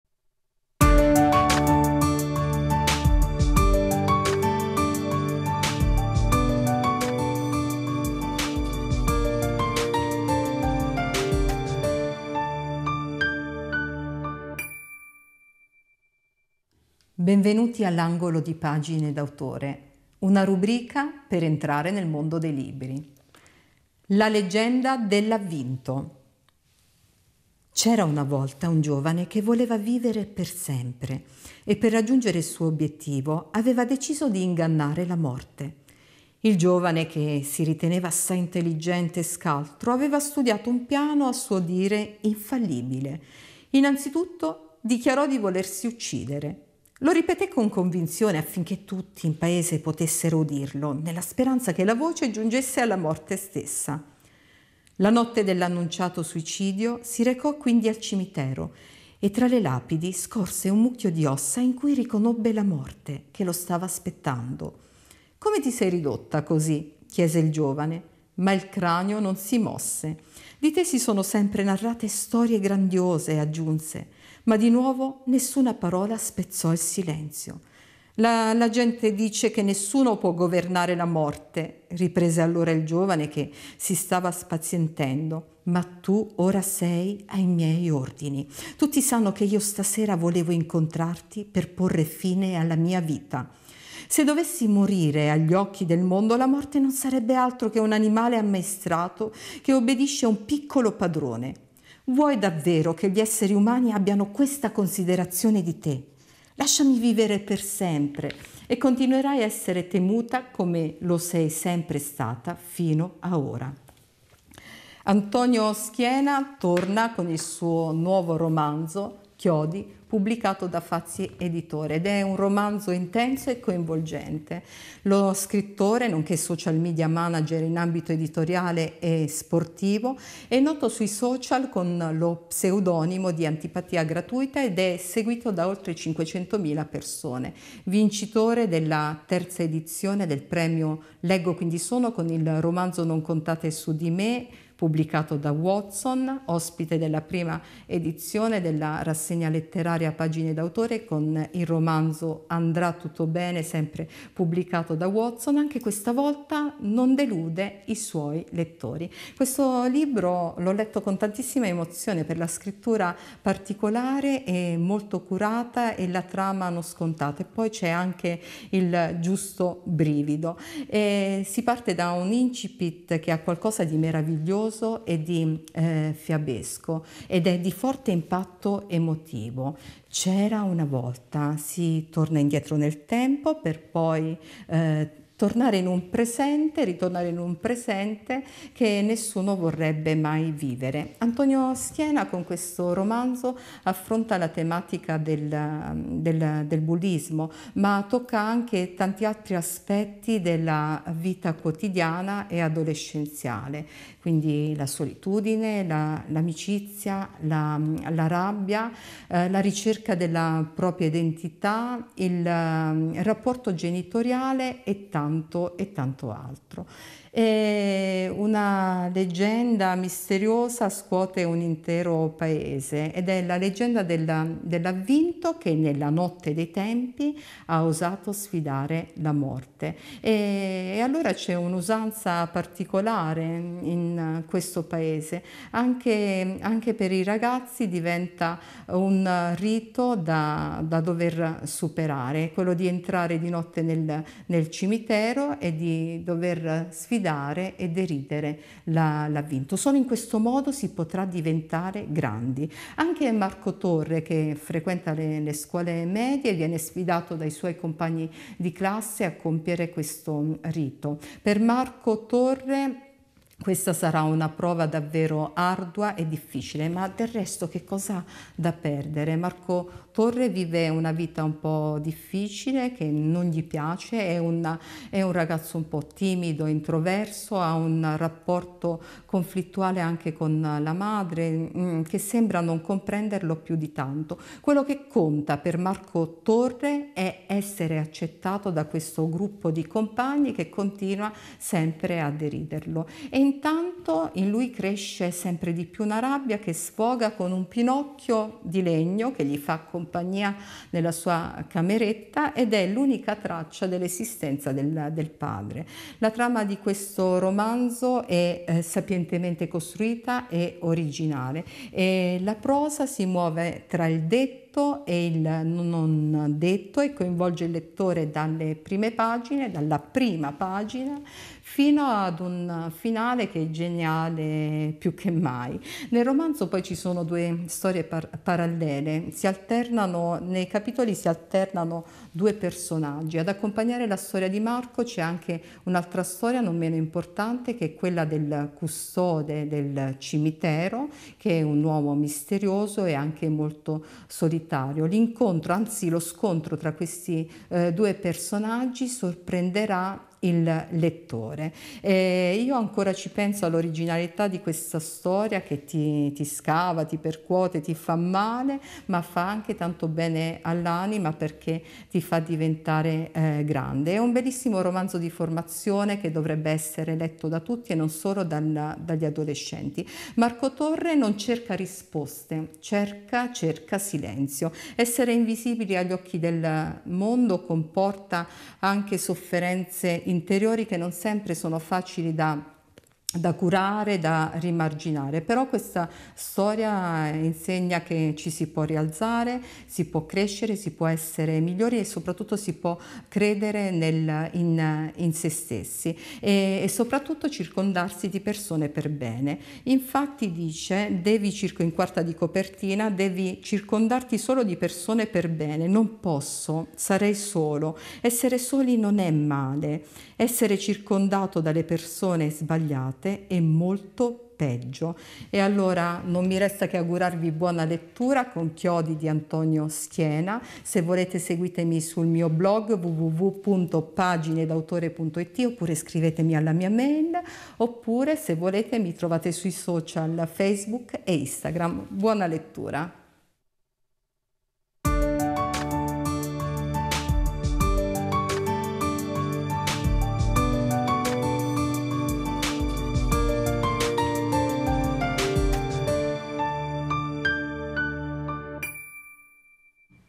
“Chiodi”, il nuovo romanzo di Antonio Schiena.
Carla Bonfitto presenta “Chiodi”, il nuovo romanzo di Antonio Schiena.